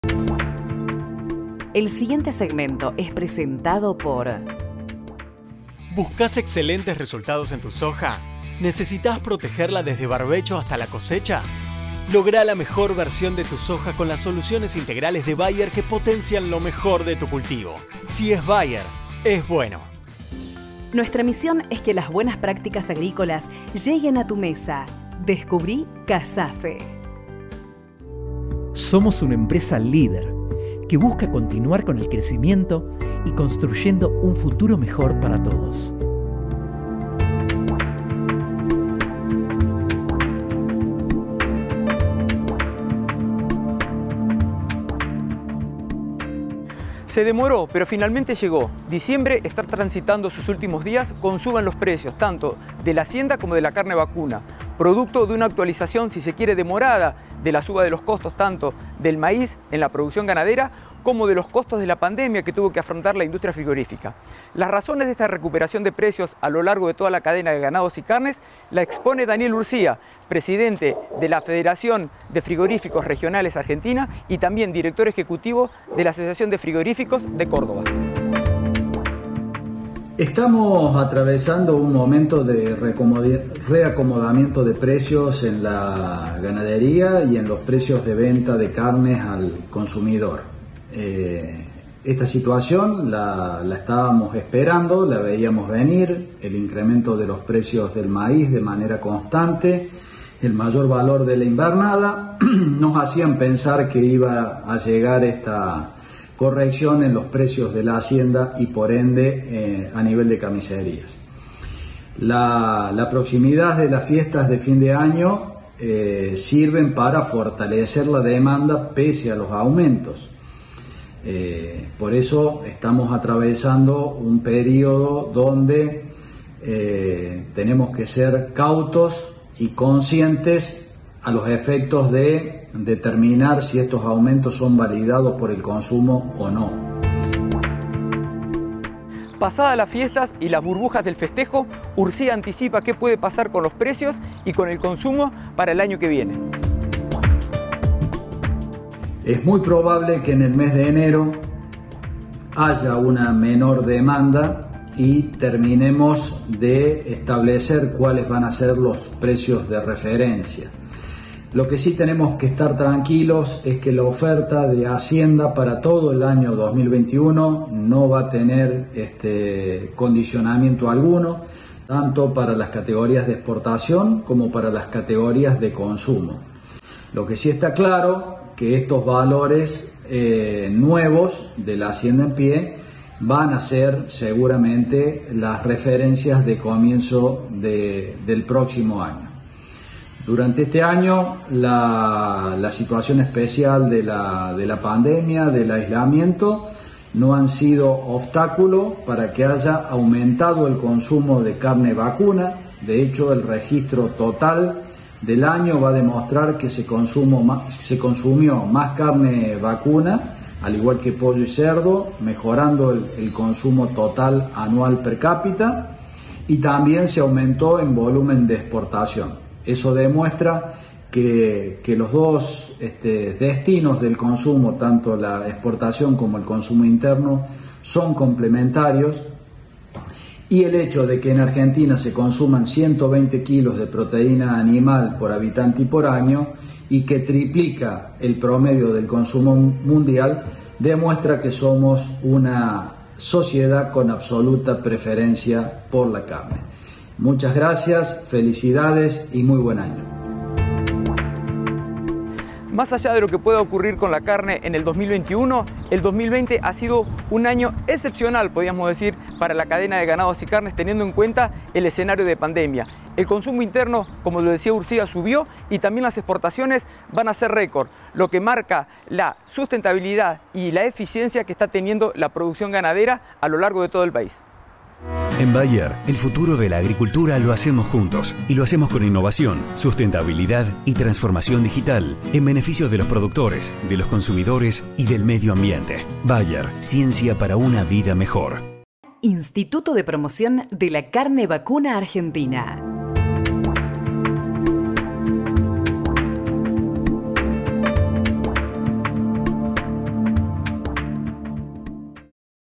Luego de las fiestas, ¿se sostendrá el precio de la carne?
Tal como se anticipaba, en este fin de año los precios de la hacienda y la carne buscan su lugar. ¿Cómo reaccionará el mercado luego del gran consumo por las fiestas?